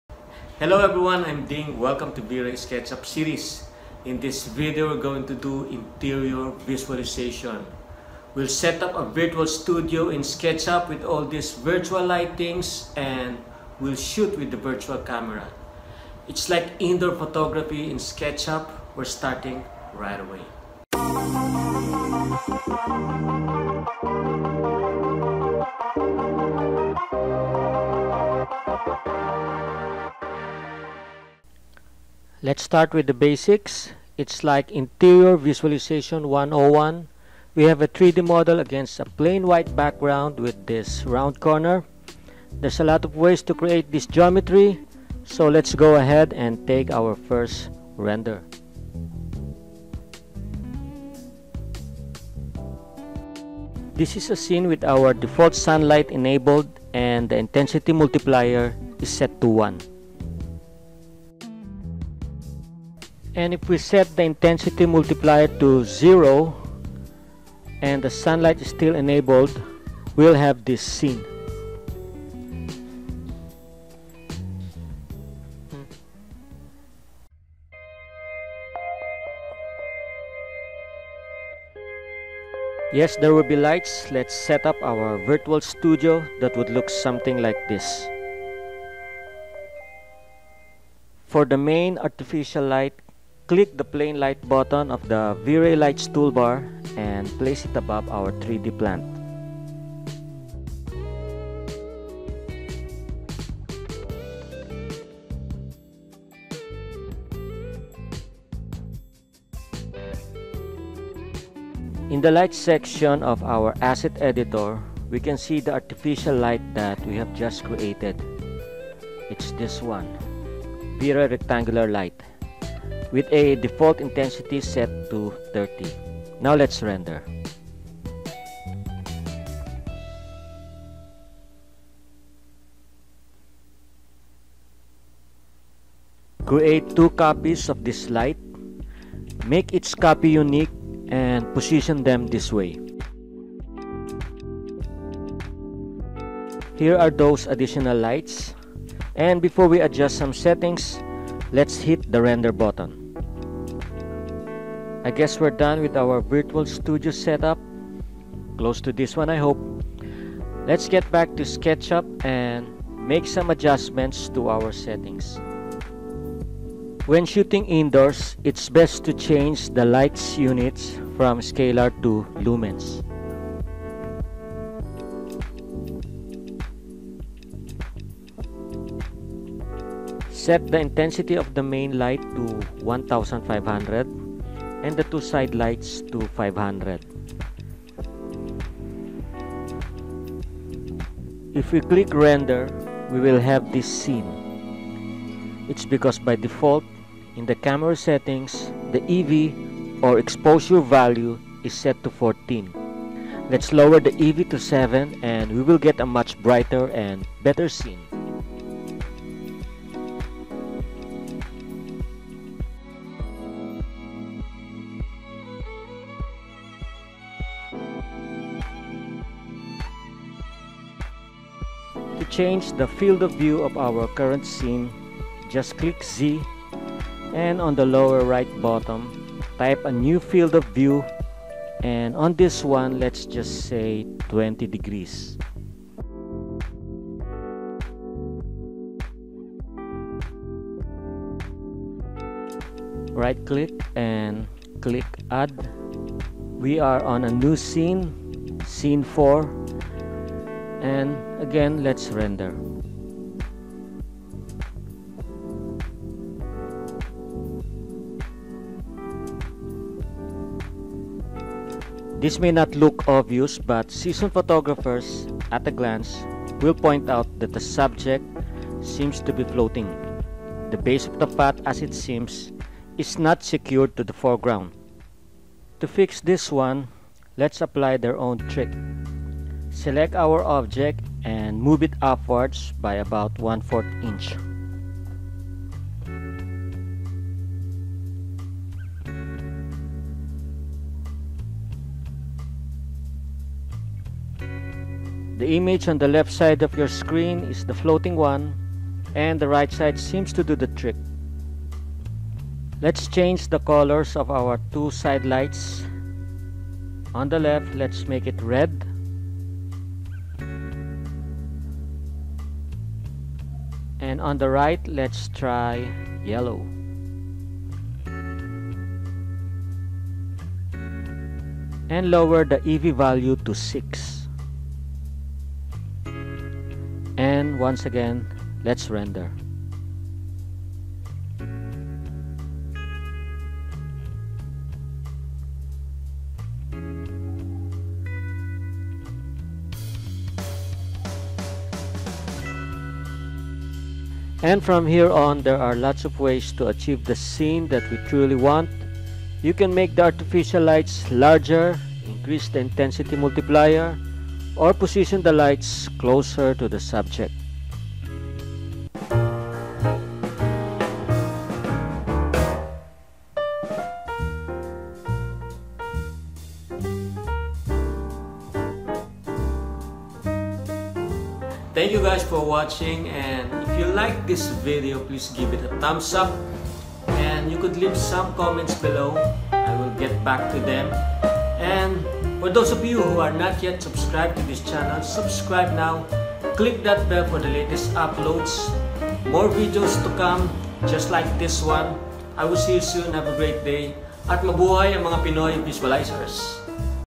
Vray For Sketchup Tutorial: Indoor Photography In Sketchup
Hello Mga Ka-Series!
Welcome to Vray Sketchup Series.  Sa video na 'to,  ipapakita ko sa inyo ang isang paraan kung paano mag interior visualizaion sa sketchup.  It will be a step by step guide.
(Pasensya na pa ulit-ulit yung mga words. Pang-SEO lang kasi)

ding_lambinicio